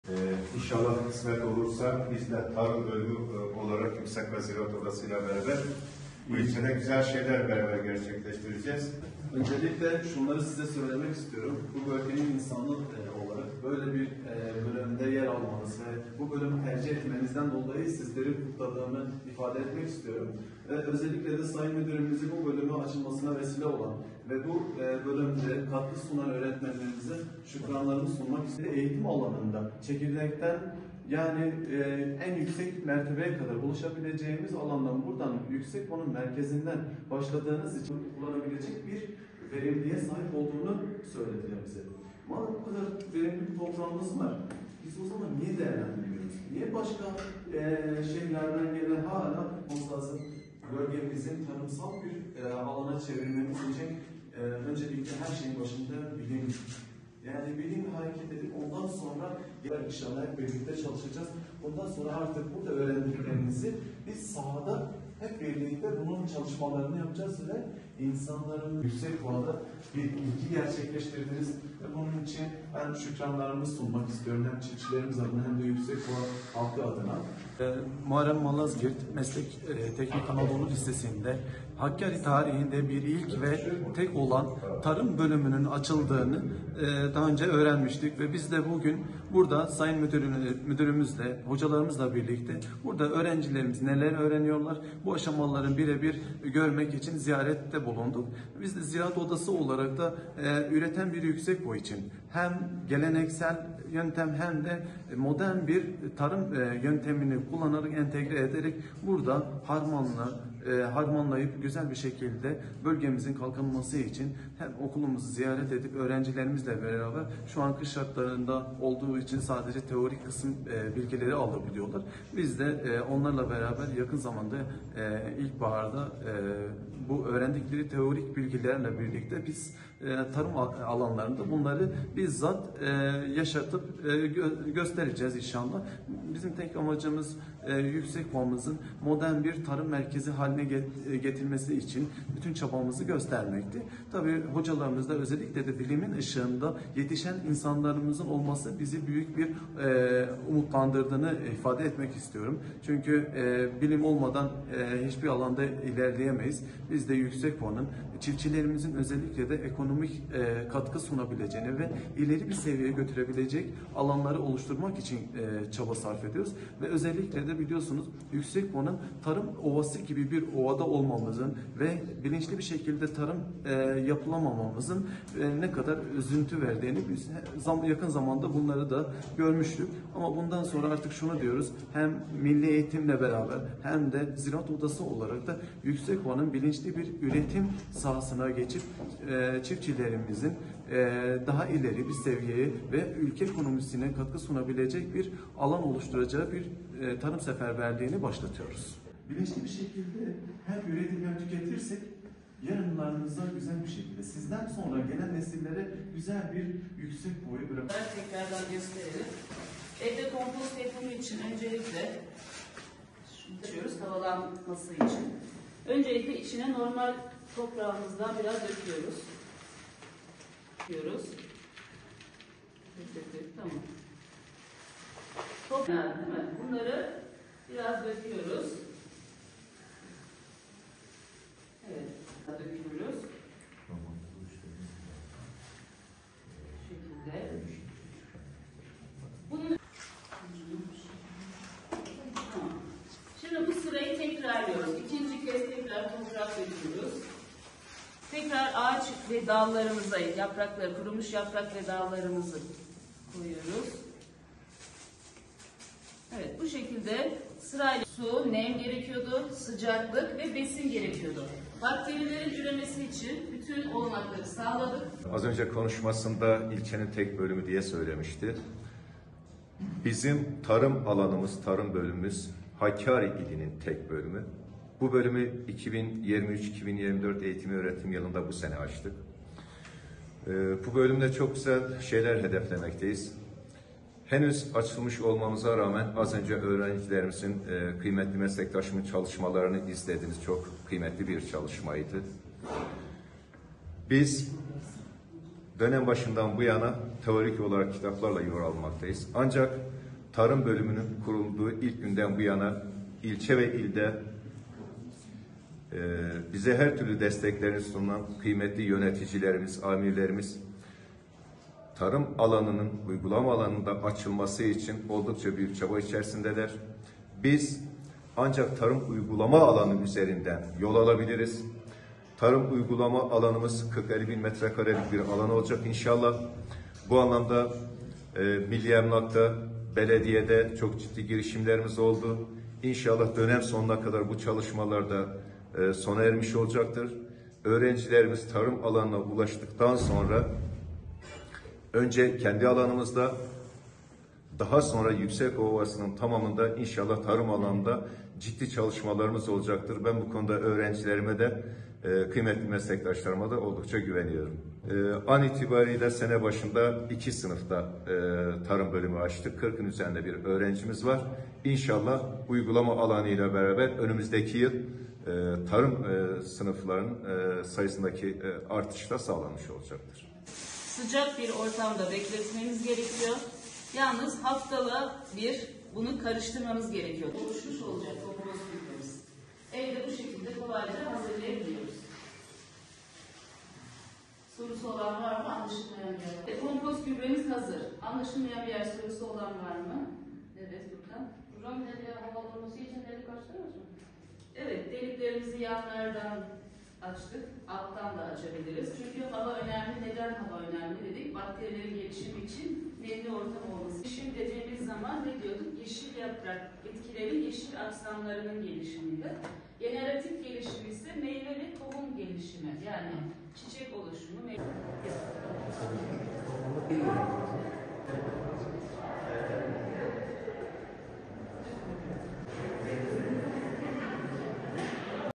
Yüksekova’da Bir İlk: 2 Tarım Sınıfı Açıldı
Hakkari’nin Yüksekova ilçesinde ‘Tarım seferberliği’ adı altında ilk kez bir okulda 2 tarım sınıfı açıldı.